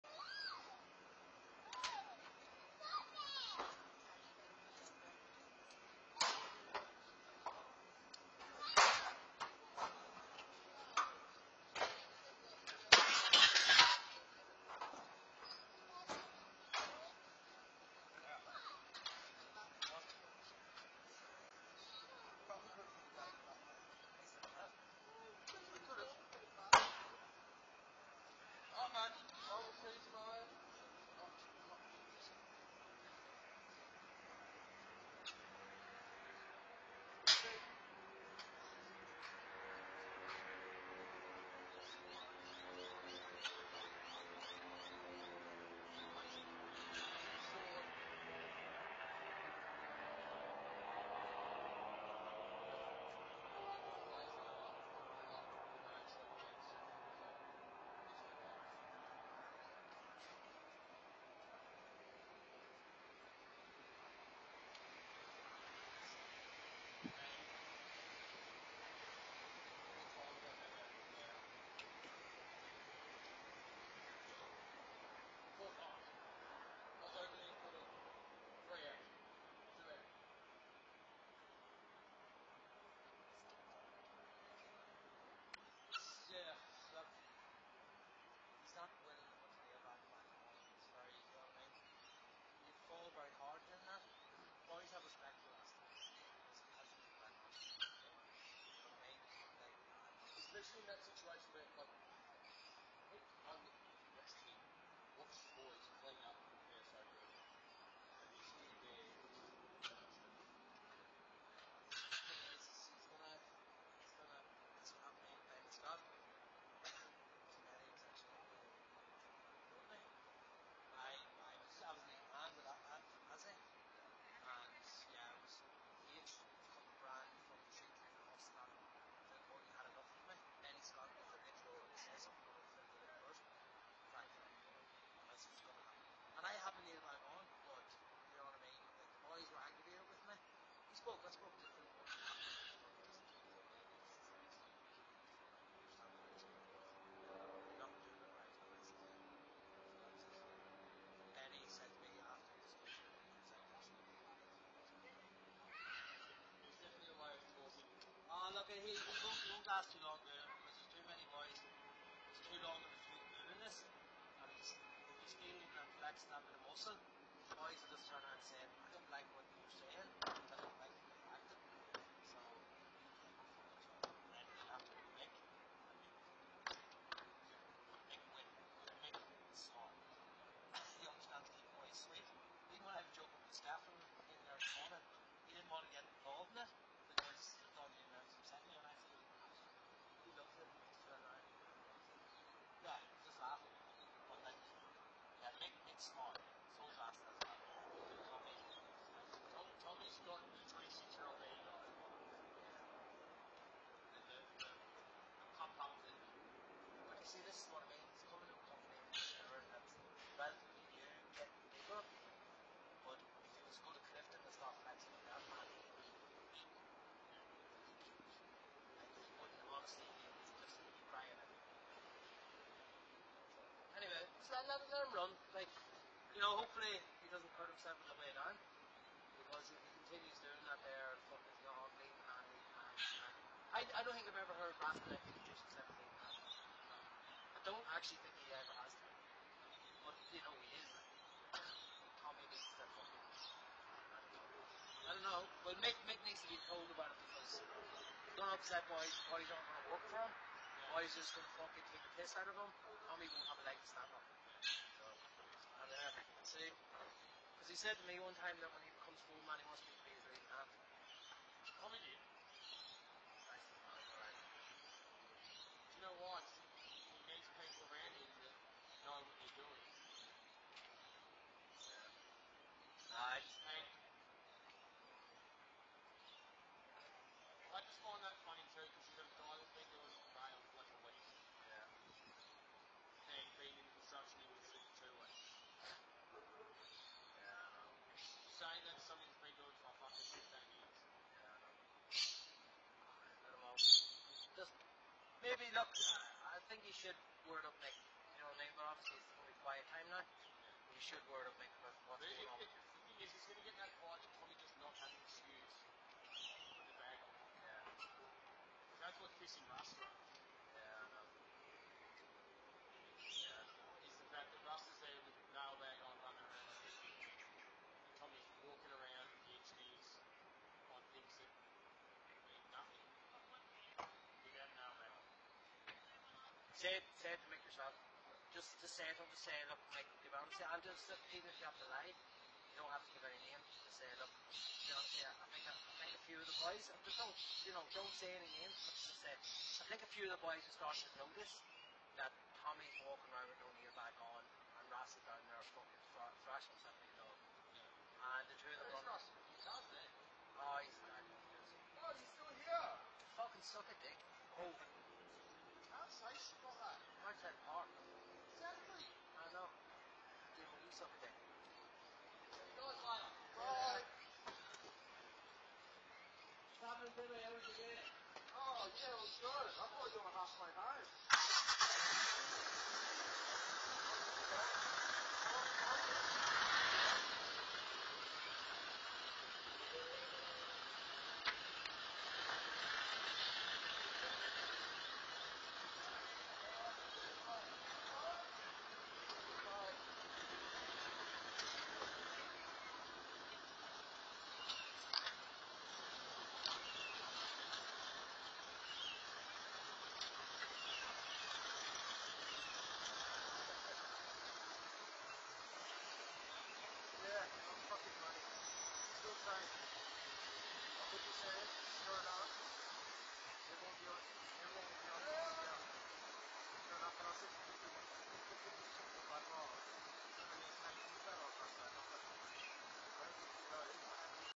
Level Crossing Worker Dumps Rubbish In Parkdale
Video shows Level Crossing Removal Project worker illegally dumping rubbish, outside a local residents house.

This ute was also parked illegally all day from 7.25am on Wed 8th May until 5:19pm.